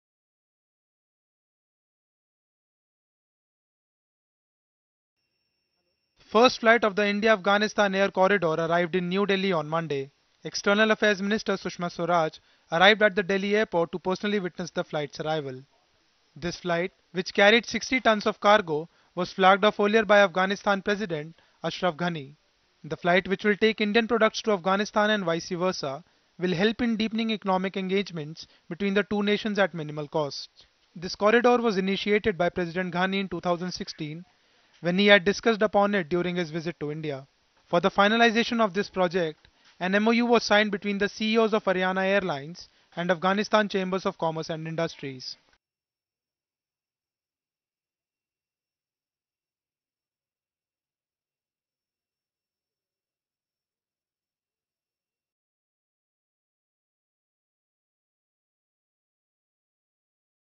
India- Afghanistan air corridor’s 1st flight arrives in New Delhi - Delhi News
air corridor arrived in New Delhi on Monday. External Affairs Minister (EAM) Sushma Swaraj arrived at the Delhi airport to personally witness the flight’s arrival. This flight, which carried 60 tonnes of cargo (mainly hing) was flagged off earlier by Afghanistan President Ashraf Ghani. The flight which will take Indian products to Afghanistan and vice versa, will help in deepening economic engagements between the two nations at minimal costs. This corridor was initiated by President Ghani in 2016 when he had discussed upon it during his visit to India. For the finalisation of this project, an MoU was signed between CEOs of Ariana Airlines and Afghanistan Chambers of Commerce and Industries (ACCI).